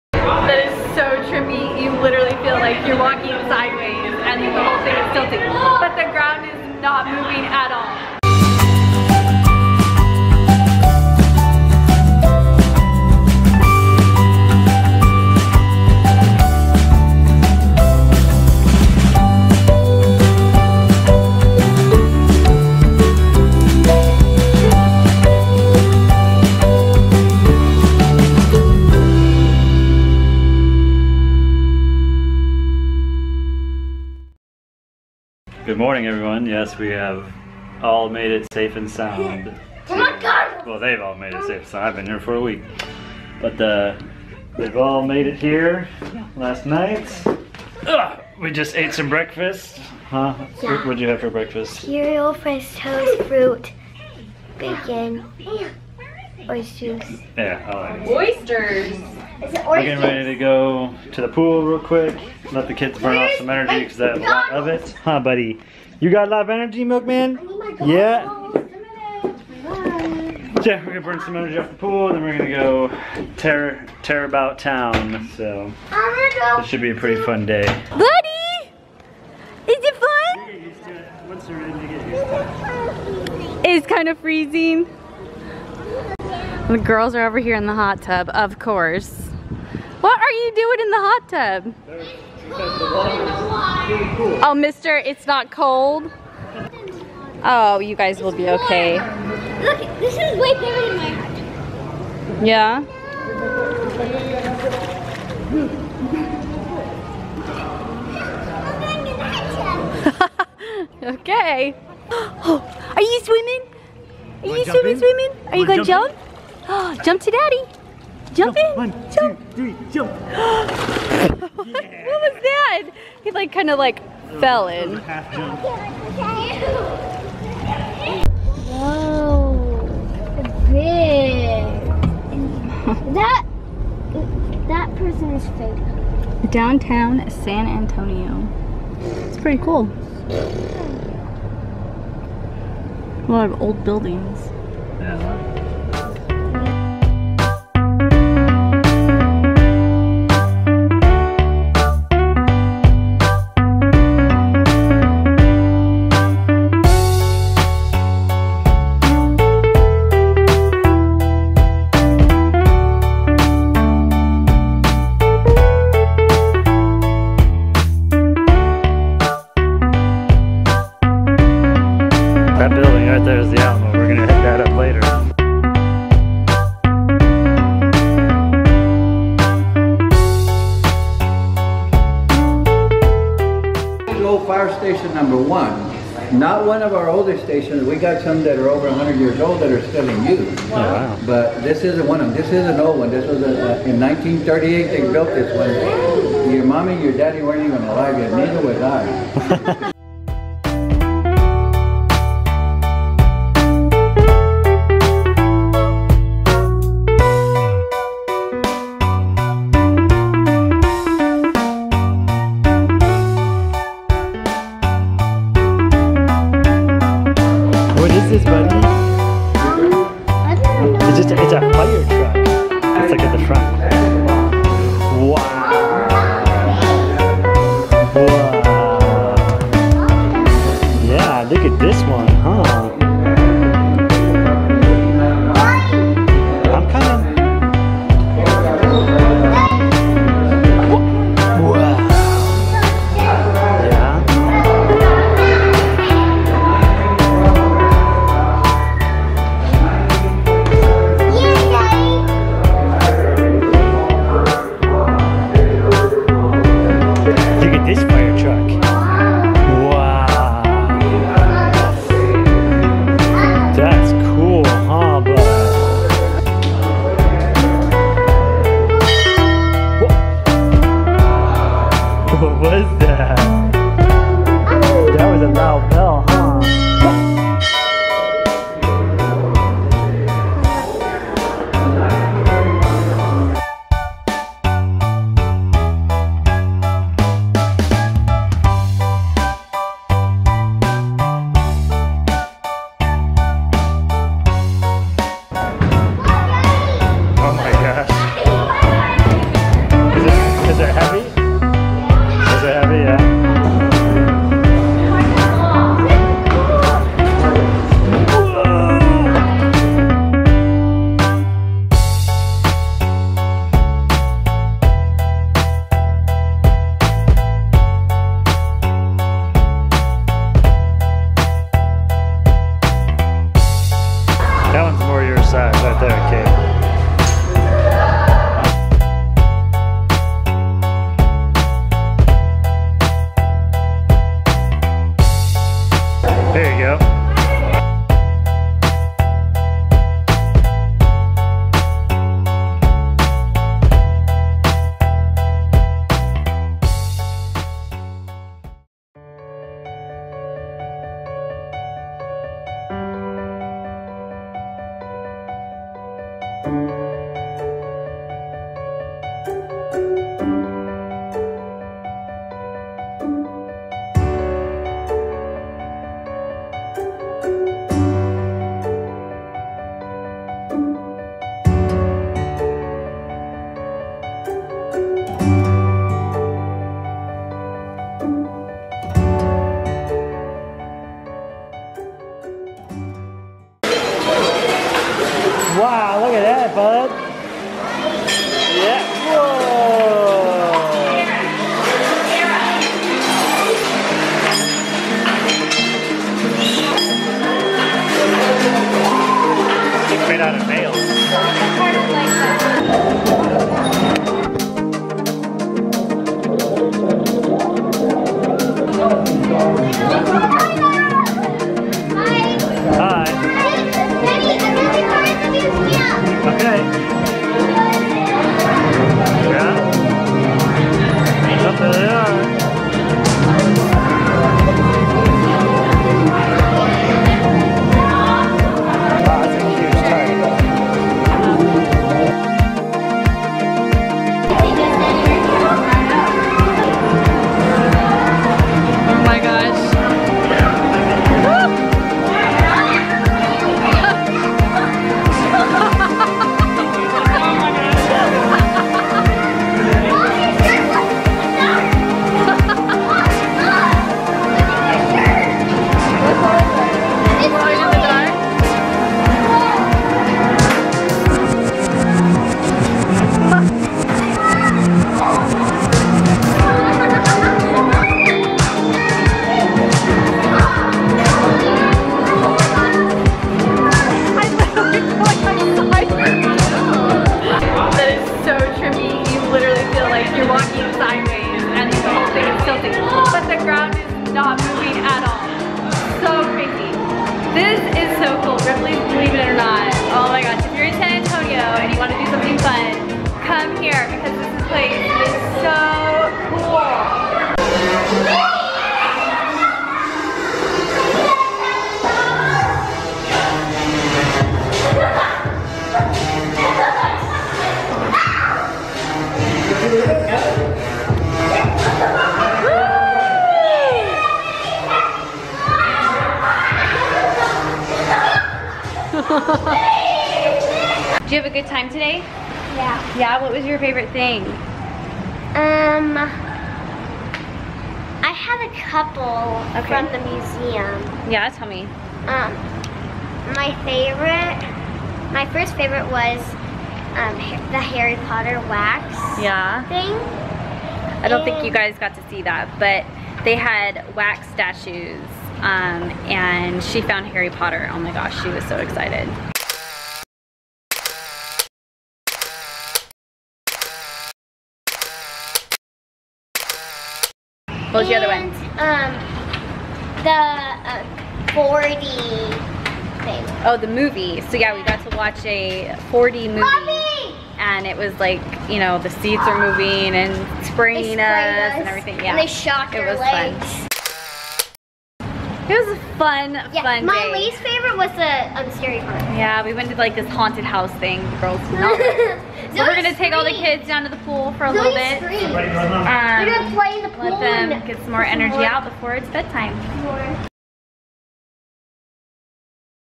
SHE MET HARRY POTTER!
We had such a great time in San Antonio over the weekend. Swimming, seeing parts of history, and experiencing Ripley's Believe It or Not was awesome! If you are ever in that area you should definitely check it out!